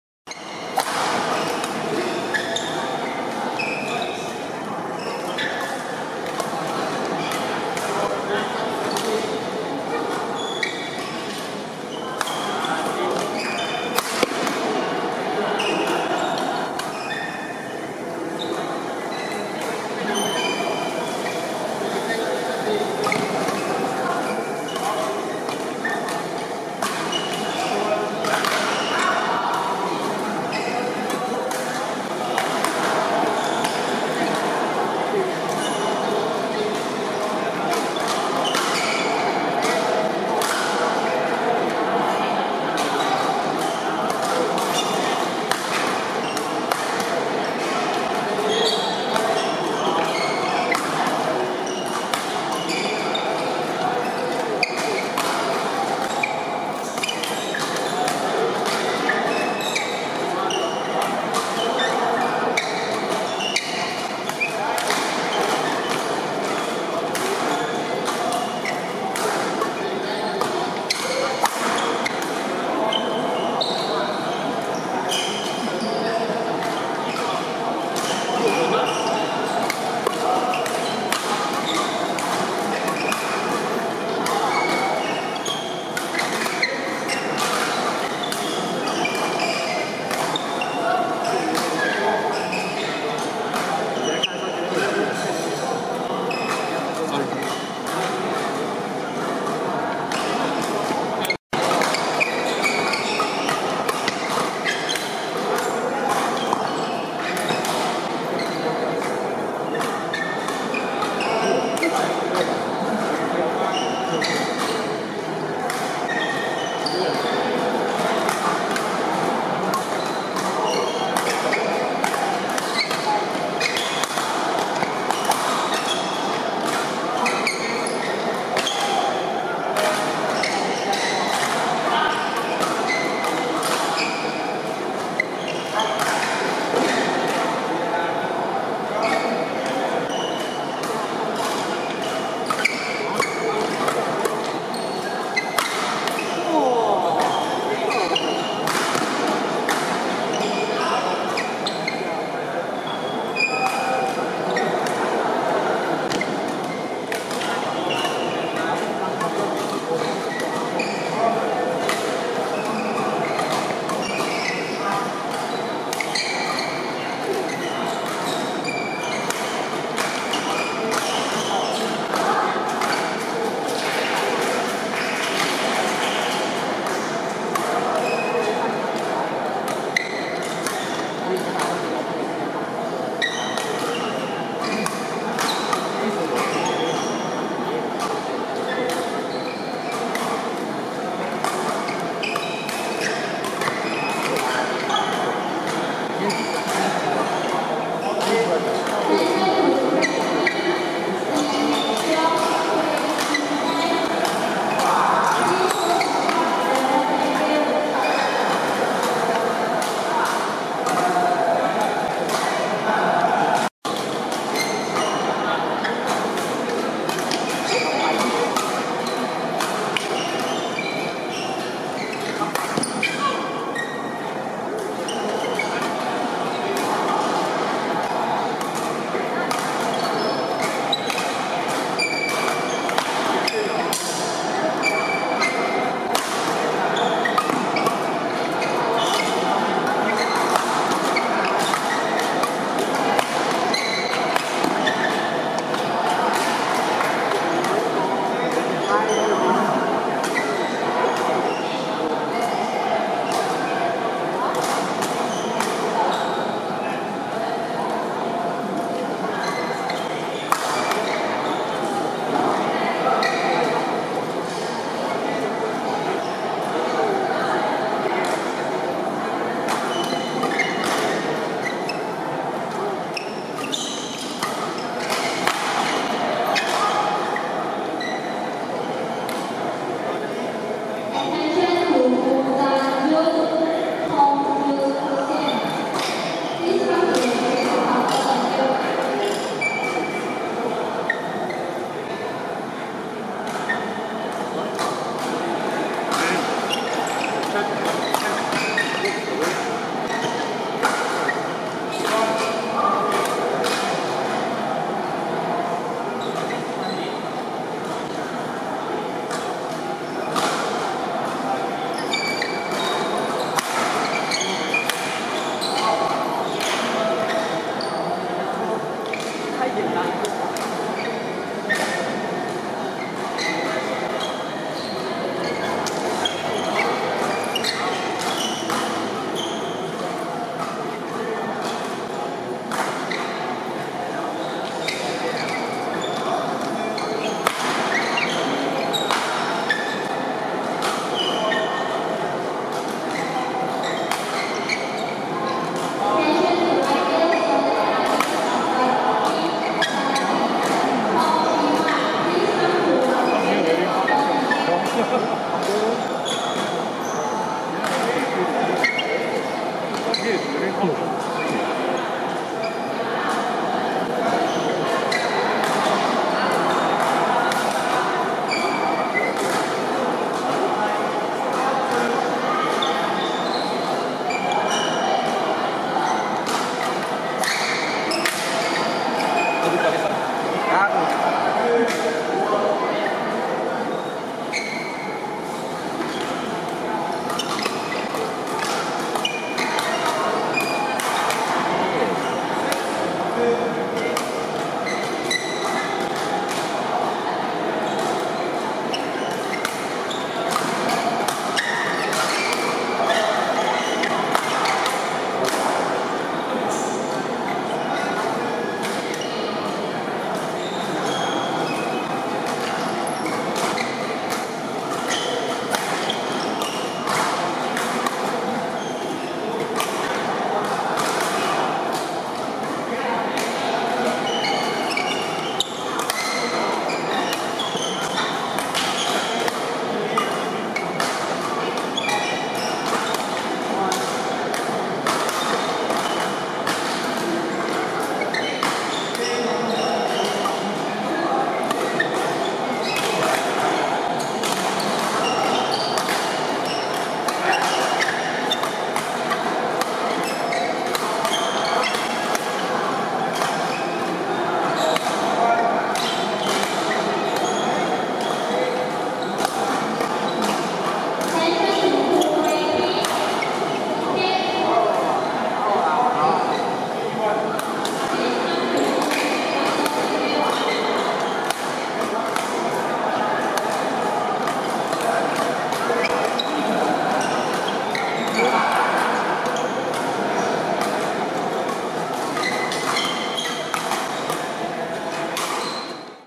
STRIVE Graded Badminton Tournament 2016 ~ XD Rd1
short video clips of STRIVE Graded Badminton Tournament 2016 ~ mixed doubles round 1

2nd Jan 2016

Venue, Ara Court, Petaling Jaya, Malaysia